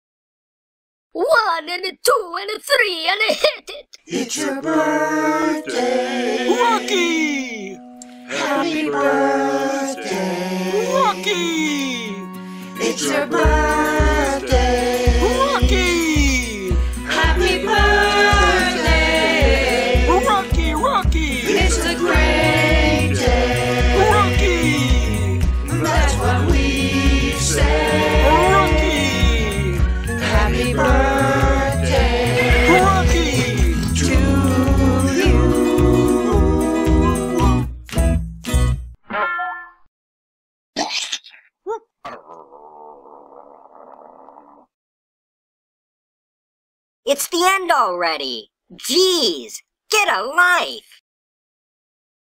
ROCKY Birthday Video 12 2013 - 12 2019
cartoon group singing customized birthday song